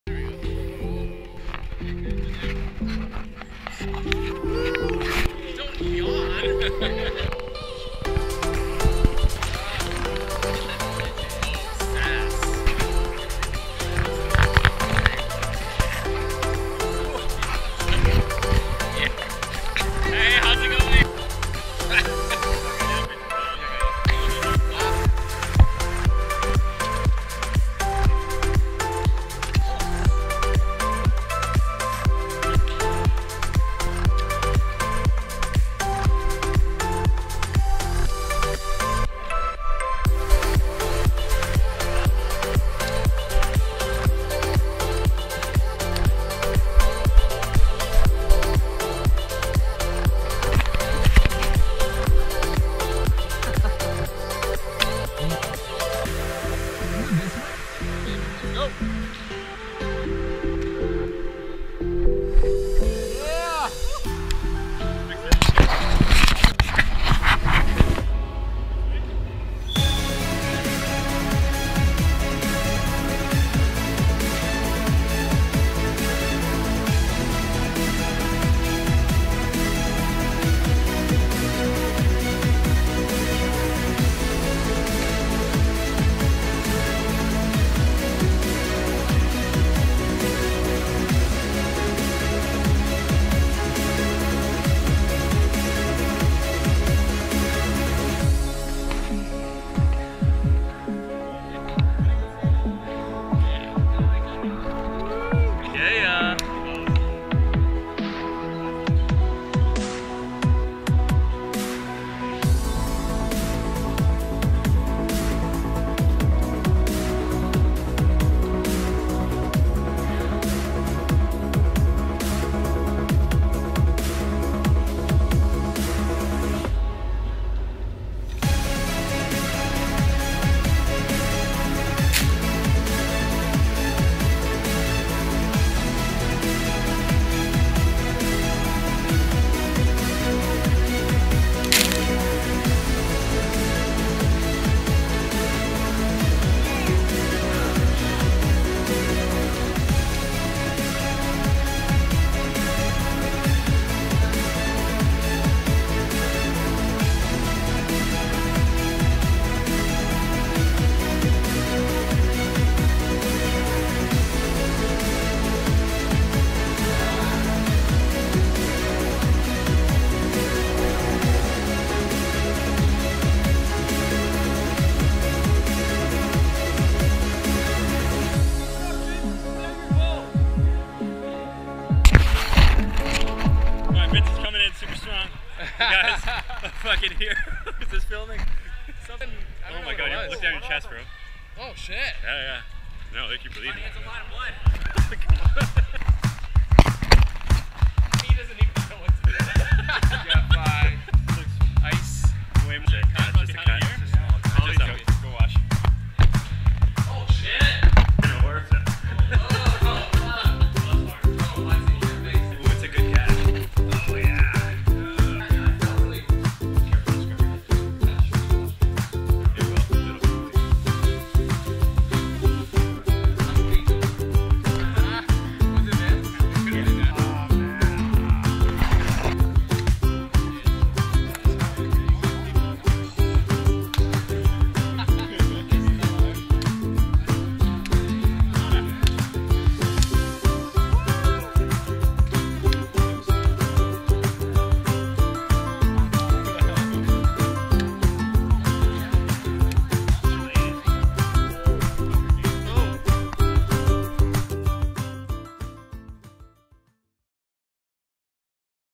The Second Voice Actor's Archery Tag - 2019
2nd bi-annual voice actors archery tag!! So great to have the voice over family shooting arrows at each other! (I caught some arrows this time!!)  Not only are these the sweetest people, but they are generous as well! The clothing drive collected a whole lot of clothes for people who really need them.  And we also had some fun of course ;)

 *Some graphic images and mature language is used.